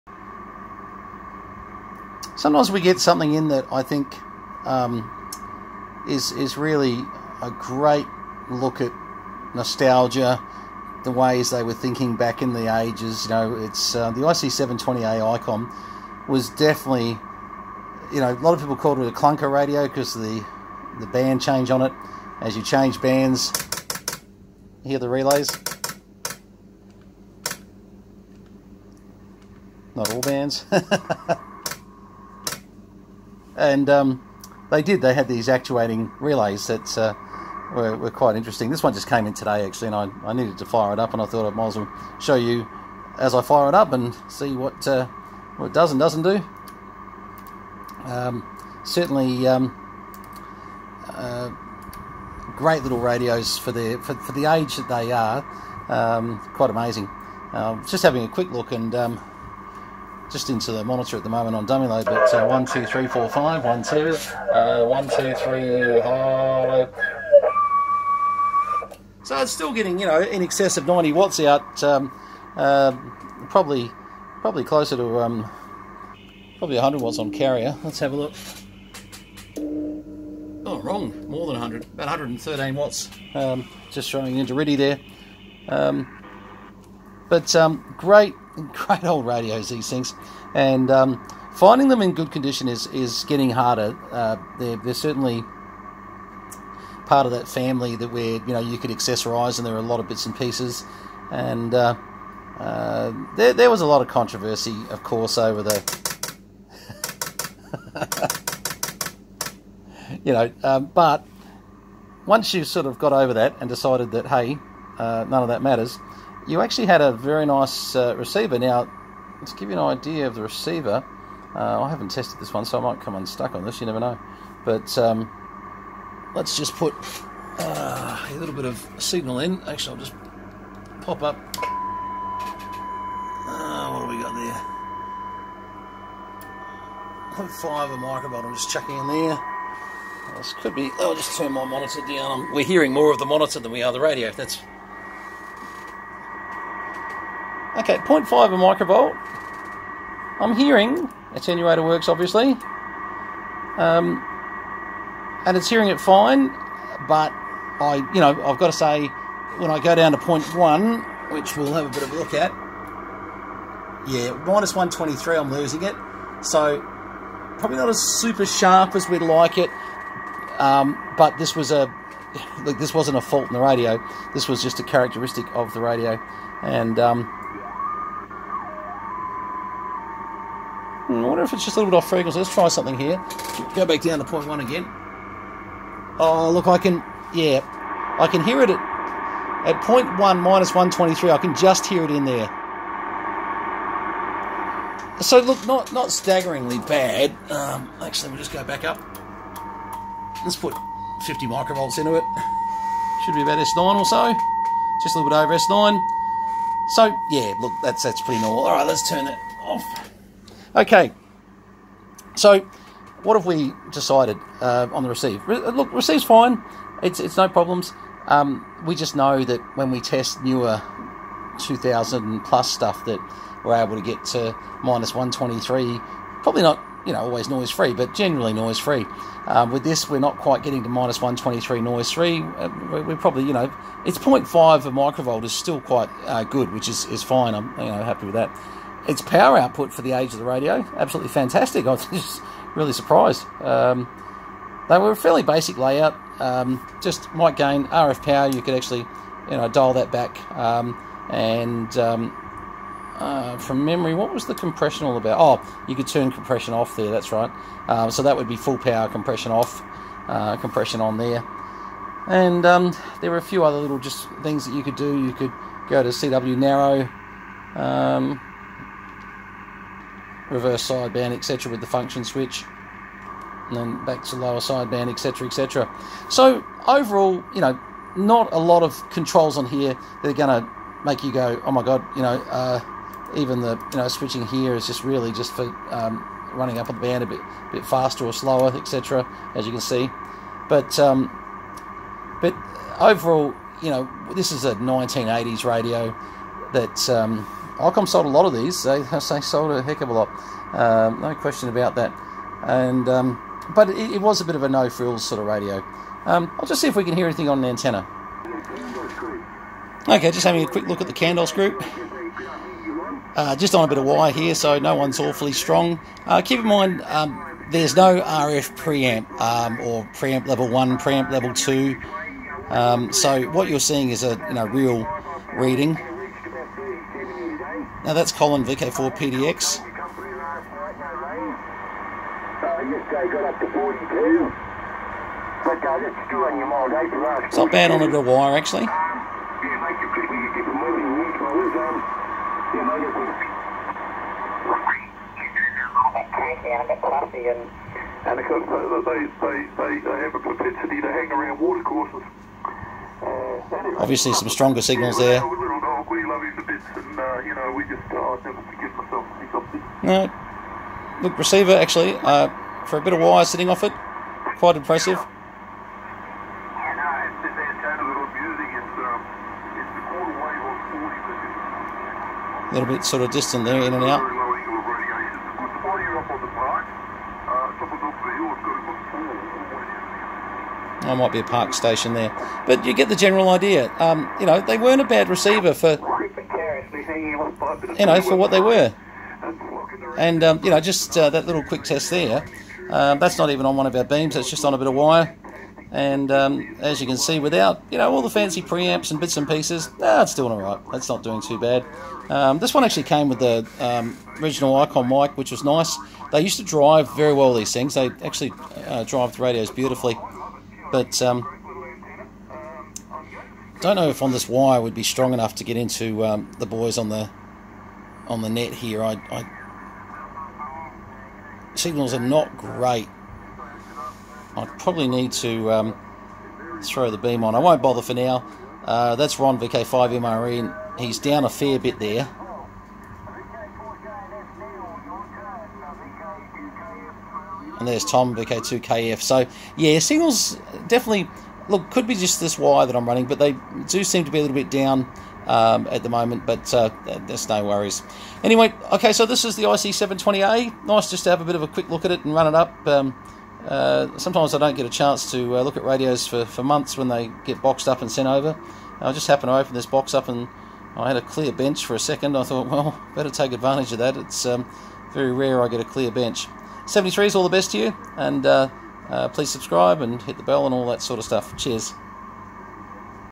Icom IC720A HF Transceiver- The Relay Clunker that works perfectly across the spectrum😁
The Icom IC720A was a highly criticized radio over the years, but surprisingly, a great little radio for a bit of HF fun. Our results show its a great little powerhouse transmitter, maybe not quite the super sensitive receiver, but also quite an adequate receiver. Overall, a great piece of historic value for sure.